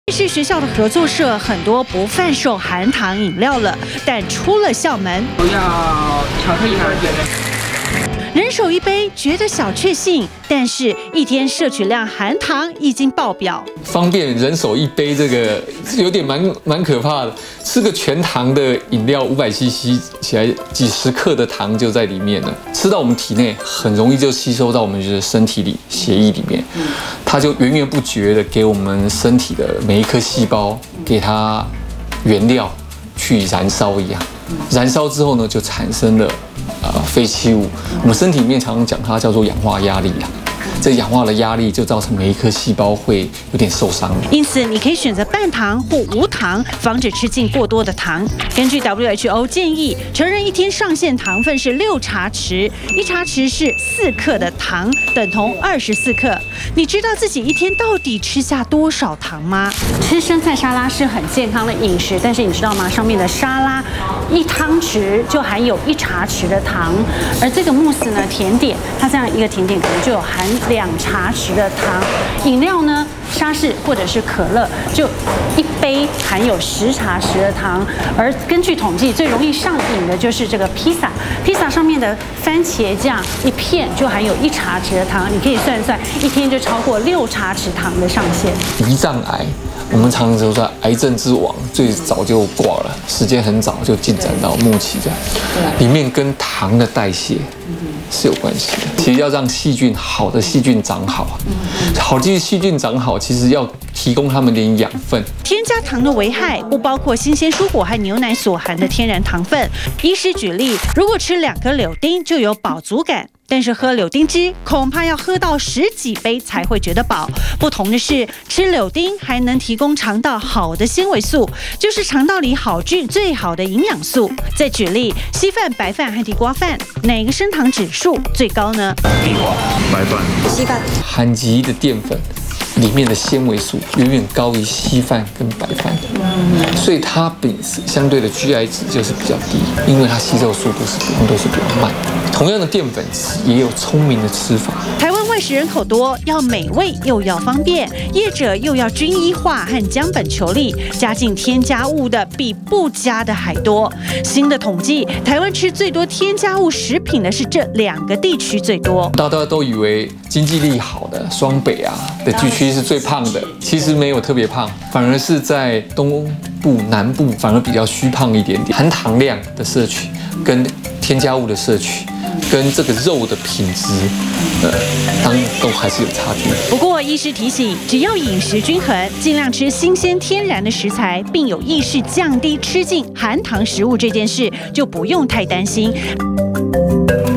20191017中天新聞 螞蟻人注意了！「嗜糖成癮」恐癌化上身、這樣吃才對
當工作、生活壓力變大，你可能不自覺吃起甜食，馬來西亞一位工程師每天至少喝上兩杯汽水飲料，不然沒有心情工作，由於攝取過多糖分，導致在工作中經常暈倒，年紀輕輕就被醫師確診為第二型糖尿病，手臂潰爛見骨、背部膿瘡，雖然術後已經把受到感染的皮膚切除，但手臂無法正常移動丟了工作。醫師提出呼籲，近年很多年輕人患了「糖上癮症」而不自知，吃過多的添加糖，包括果糖、代糖、白糖的食品，對身體細胞癌化的風險！提醒必須有意識去減少、減量吃糖這件事。